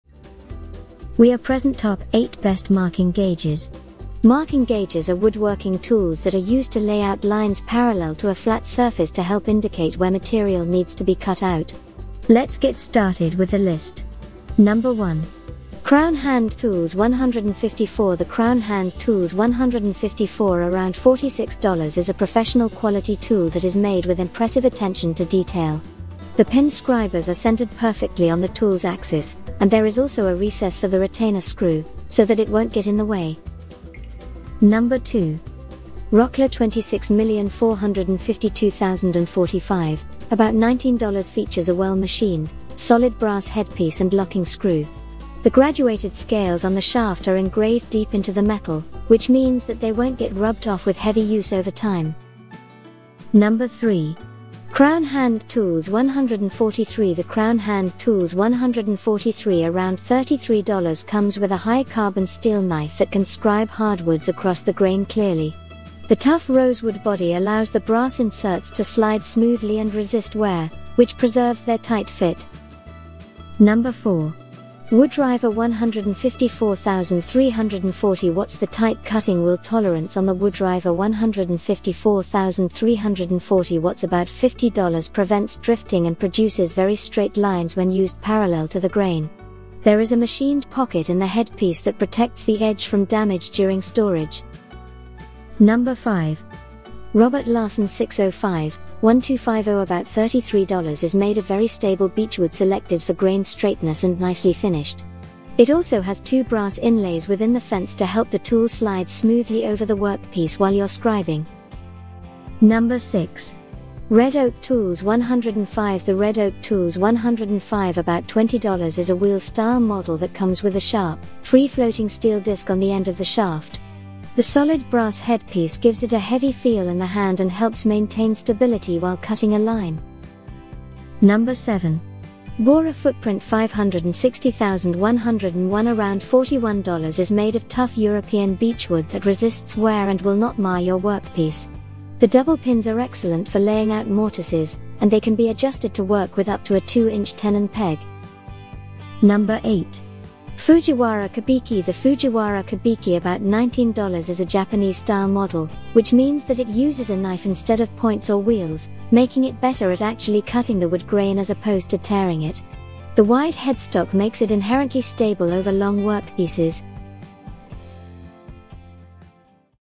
Top 8 Best Marking Gauges 2020
We are present Top 8 Best Marking Gauges.  Marking gauges are woodworking tools that are used to lay out lines parallel to a flat surface to help indicate where material needs to be cut out.
Lets get started with the list.

1. Crown Hand Tools 154
2. Rockler 26452045
3. Crown Hand Tools 143
4. Woodriver 154340W
5. Robert Larson 605-1250
6. Red Oak Tools 105
7. Bora Footprint 560101
8. Fujiwara Kebiki